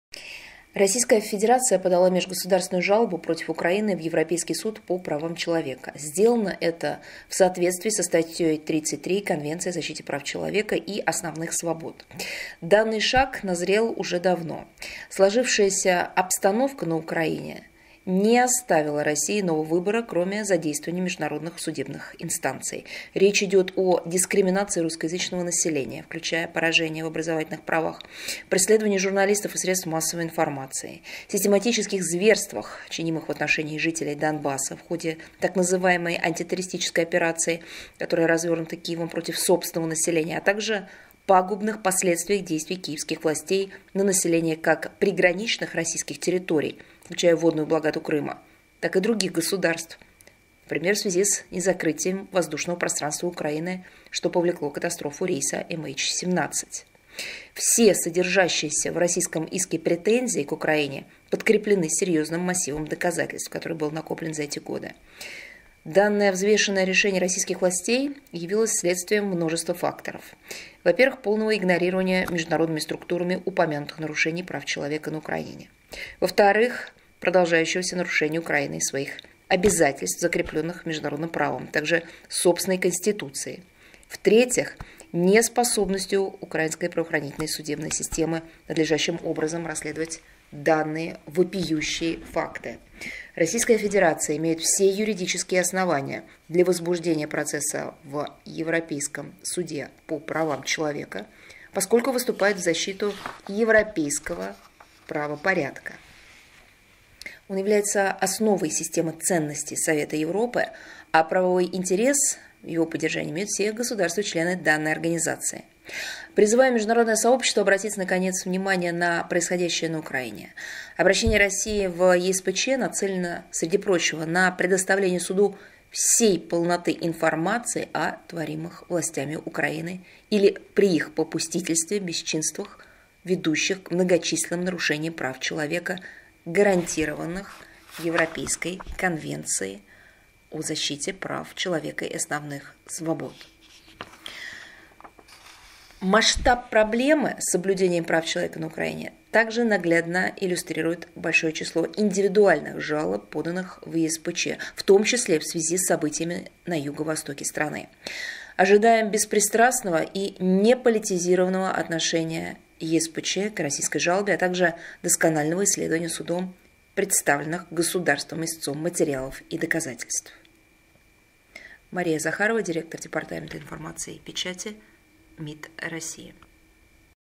Жалоба против Украины в Европейский Суд по правам человека
Российская Федерация подала межгосударственную жалобу против Украины в Европейский Суд по правам человека (ЕСПЧ) в соответствии со статьей 33 Конвенции о защите прав человека и основных свобод (ЕКПЧ).

Данный шаг назрел уже давно. Сложившаяся обстановка на Украине не оставила России иного выбора, кроме задействования международных судебных инстанций. Речь идет о дискриминации русскоязычного населения, включая поражение в образовательных правах; преследовании журналистов и СМИ, систематических зверствах, чинимых в отношении жителей Донбасса в ходе так называемой антитеррористической операции, развернутой Киевом против собственного населения, а также пагубных последствиях действий киевских властей на население как приграничных российских территорий, включая водную блокаду Крыма, так и других государств – в связи с незакрытием воздушного пространства Украины, повлекшим катастрофу рейса МН-17. Все содержащиеся в российском иске претензии к Украине подкреплены серьезным массивом доказательств, который был накоплен за эти годы.

Данное взвешенное решение российских властей явилось следствием множества факторов: во-первых, полного игнорирования международными структурами упомянутых нарушений прав человека на Украине; во-вторых, продолжающегося нарушения Украиной своих обязательств, закрепленных международным правом, а также собственной Конституцией; в-третьих, неспособностью украинской правоохранительной и судебной системы надлежащим образом расследовать данные вопиющие факты.

Российская Федерация имеет все юридические основания для возбуждения процесса в ЕСПЧ, поскольку выступает в защиту европейского правопорядка. Он является основой системы ценностей Совета Европы, а правовой интерес в его поддержании имеют все государства-члены данной организации.

Призываем международное сообщество обратить, наконец, внимание на происходящее на Украине. Обращение России в ЕСПЧ нацелено, среди прочего, на предоставление Суду всей полноты информации о творимых властями Украины или при их попустительстве бесчинствах, ведущих к многочисленным нарушениям прав человека, гарантированных ЕКПЧ.

Масштаб проблемы с соблюдением прав человека на Украине также наглядно иллюстрирует большое число индивидуальных жалоб, поданных в ЕСПЧ, в том числе в связи с событиями на юго-востоке страны.

Ожидаем беспристрастного и неполитизированного отношения ЕСПЧ к российской жалобе, а также досконального исследования Судом представленных государством-истцом материалов и доказательств.